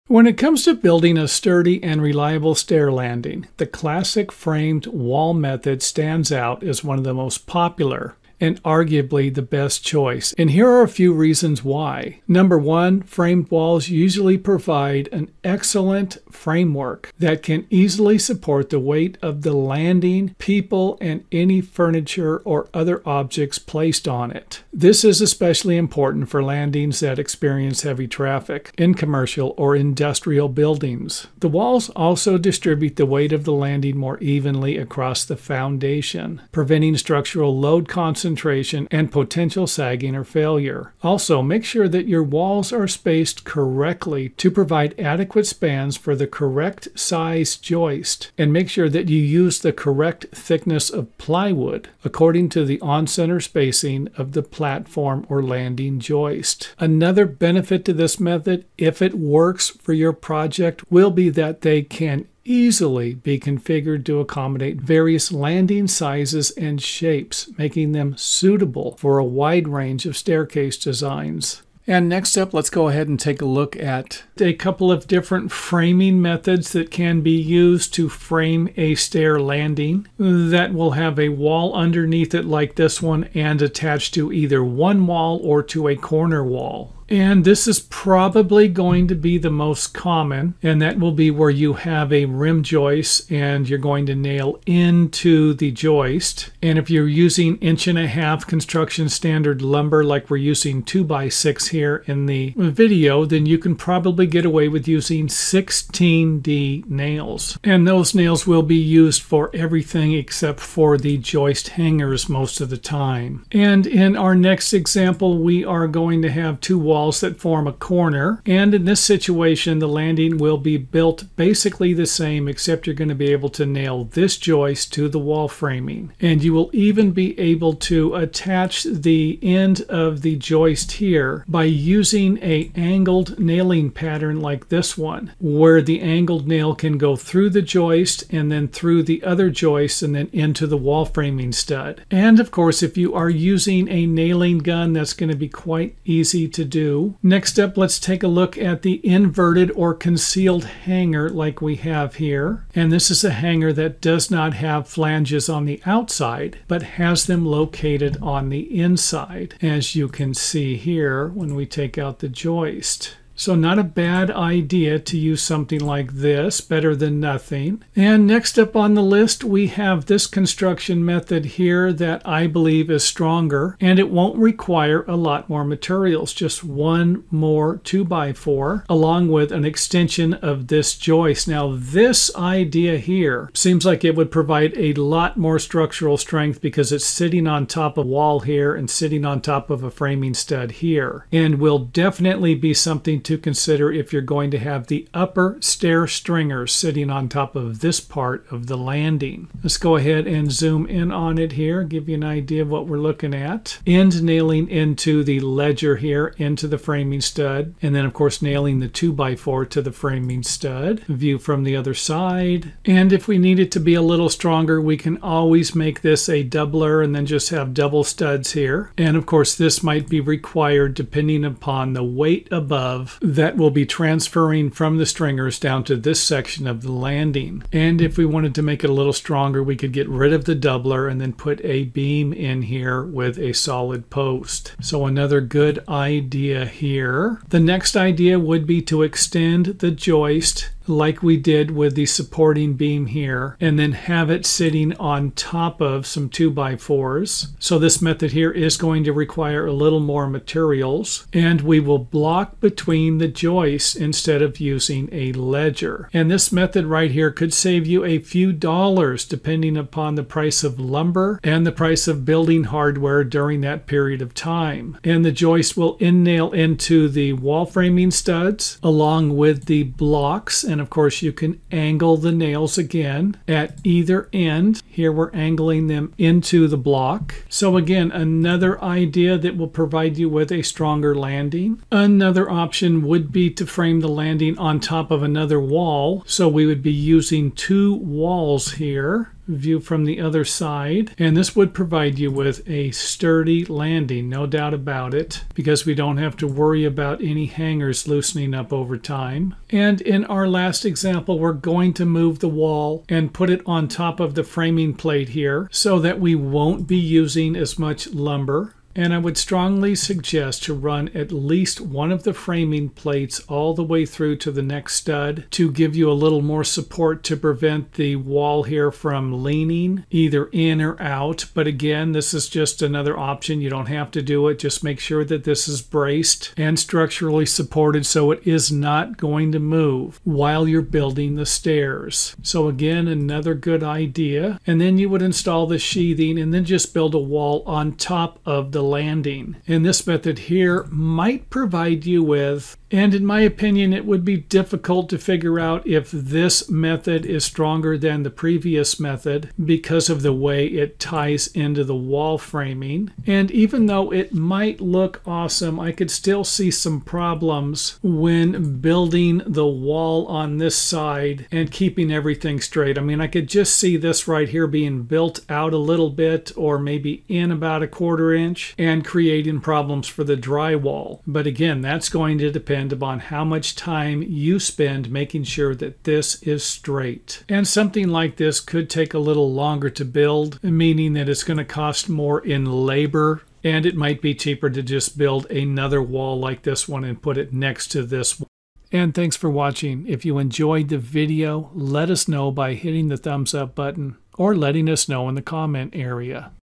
Carpentry Tips - Comparing Different Stair Landing Support Methods
- If you're building a staircase, the landing is a critical structural component that needs to be strong, stable, and built to last. In this video, we'll show you why using framed walls for support is one of the best and most reliable methods for constructing a stair landing.

First, we'll cover the key benefits of the framed wall method, including its superior ability to support heavy loads, distribute weight evenly across the foundation, and prevent sagging over time.

Then, we dive into the details, showcasing a variety of professional framing techniques you can use depending on your specific layout. You will learn how to:

Properly attach a landing to a single wall or into a corner using a rim joist.

Effectively use nailing patterns, including angled nailing for secure corner connections.

Work with different types of hardware like standard and inverted (concealed) joist hangers.

Implement stronger support methods by having joists bear directly on wall framing instead of relying only on fasteners.

Incorporate blocking between joists as a cost-effective alternative to ledgers and hangers.

Evaluate the pros and cons of building a landing on top of a full support wall versus building the wall on top of the landing platform.